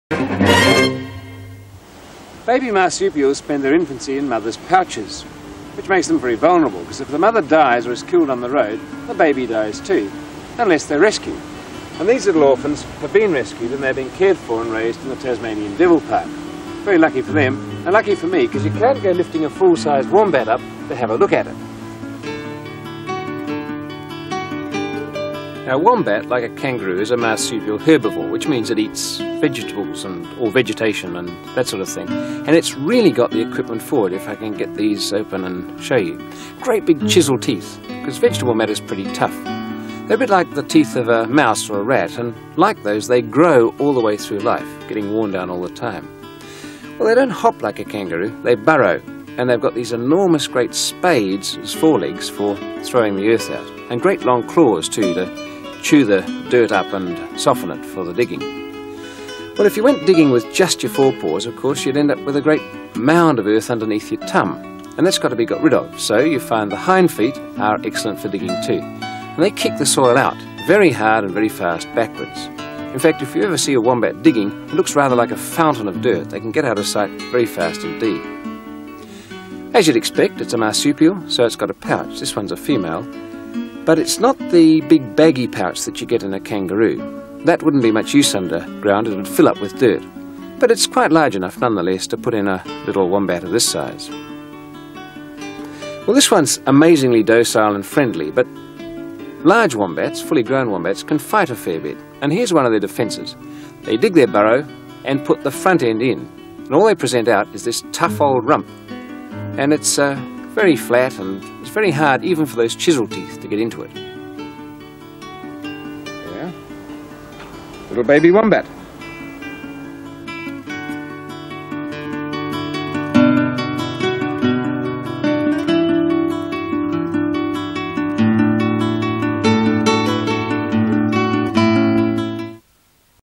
Meet Australia's Wonderful Wombat
The Australian wombat is often seen as a slightly comical marsupial but, as Rob explains, these lovely creatures are superbly adapted to their digging, herbivorous life.